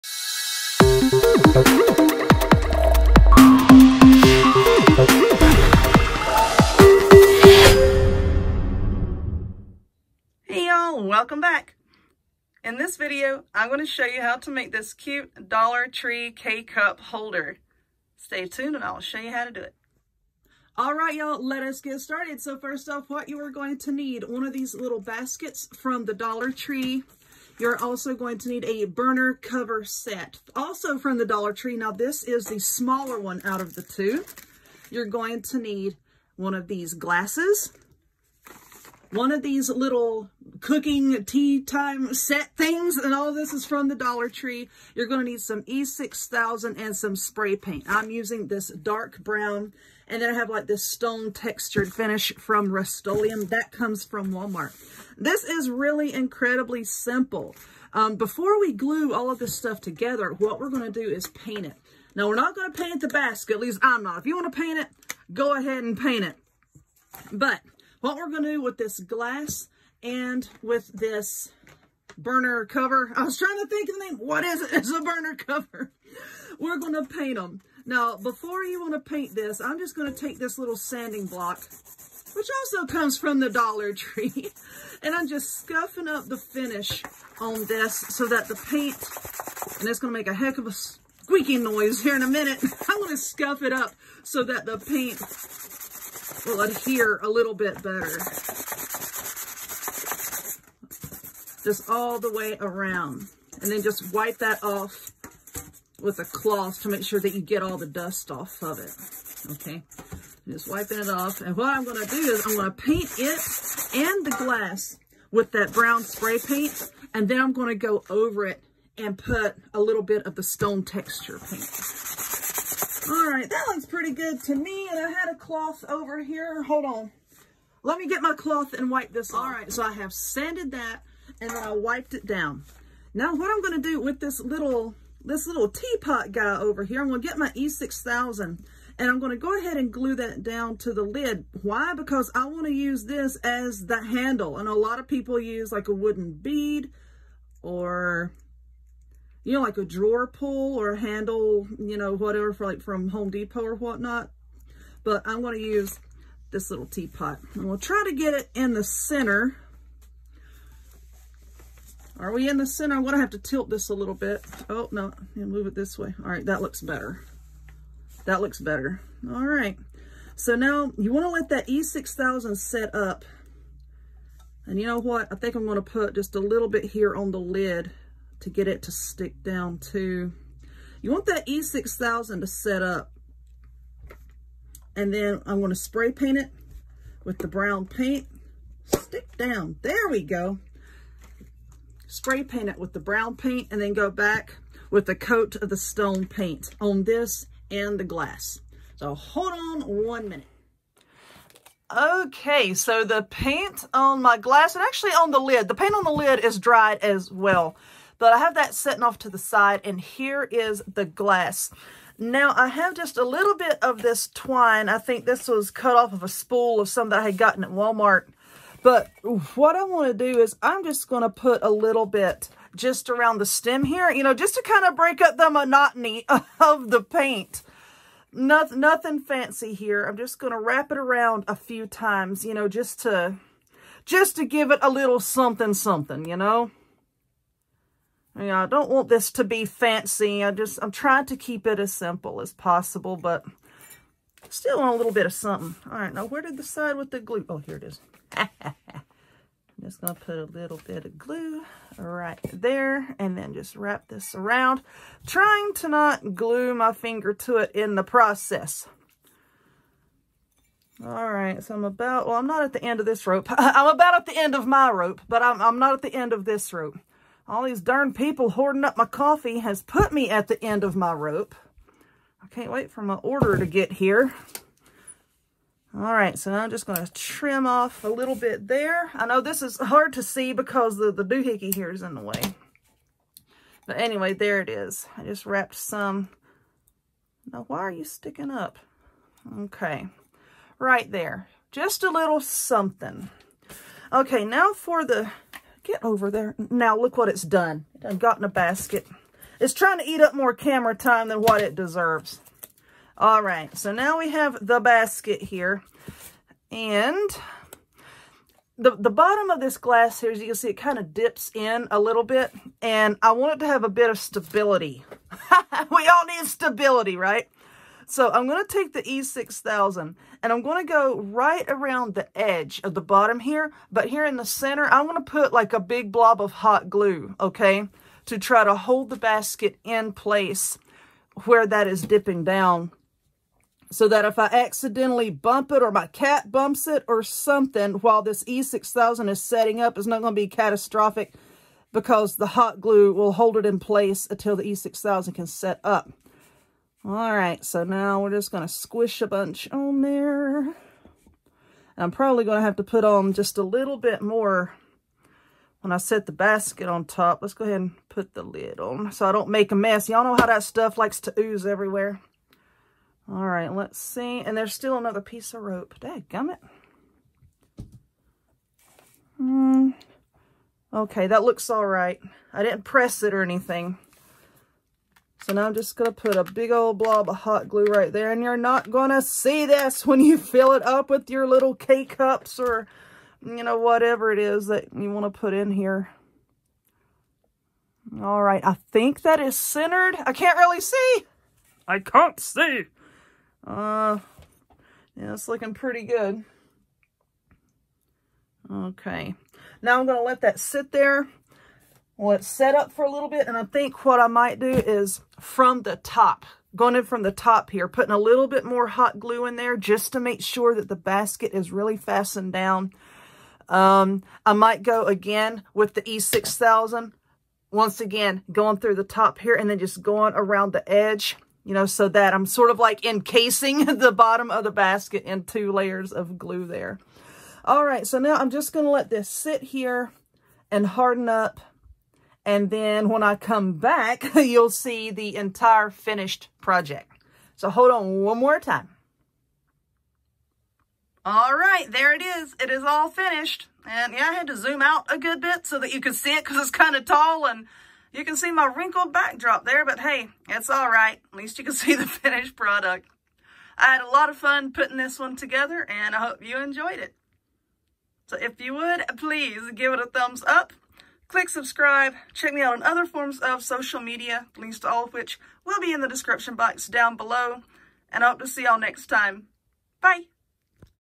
Everyday Crafting: Easy Dollar Tree K-Cup Holder
Hey y’all! In this video, I’m going to show you how to make this easy Dollar Tree k cup holder.

My favorite crafting supplies

Have you heard of Chalk Couture?! Check it out!

Send me some Friend Mail!